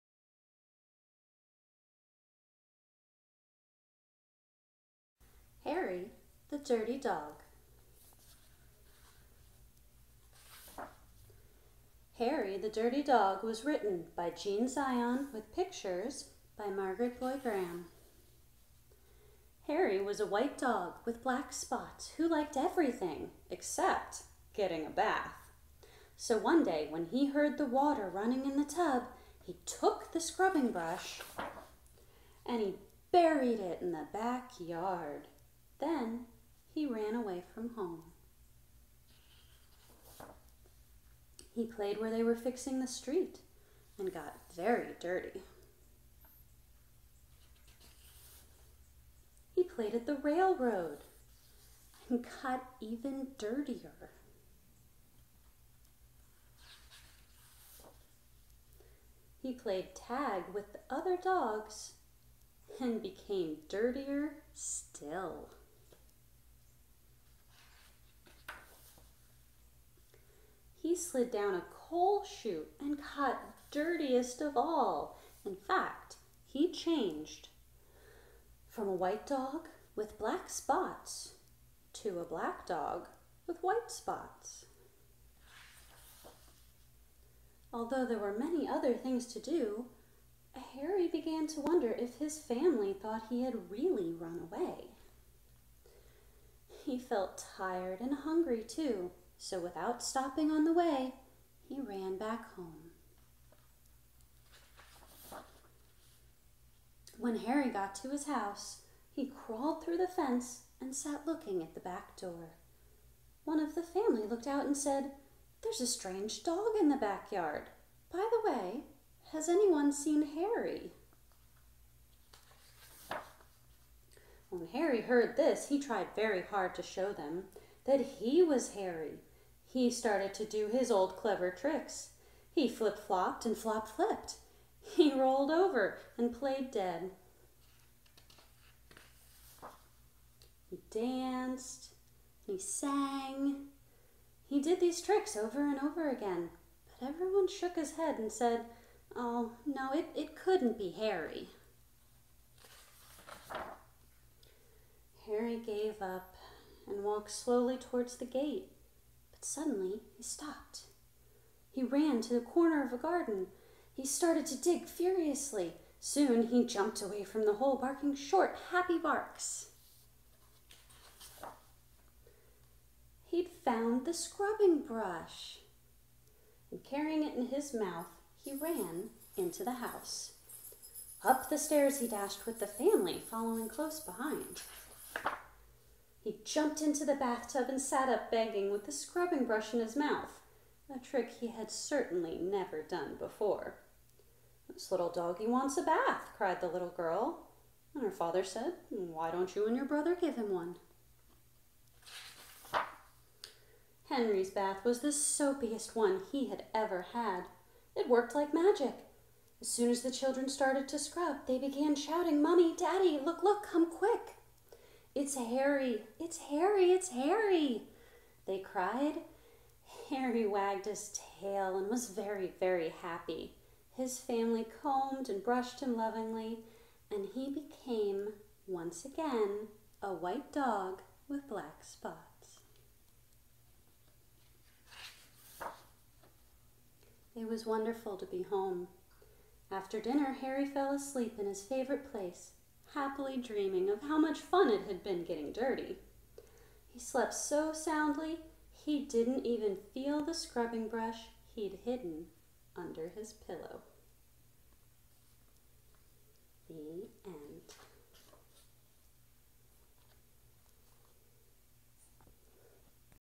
Roaming Rosie Reads Harry the Dirty Dog
Harry the Dirty Dog is by Gene Zion.

for more!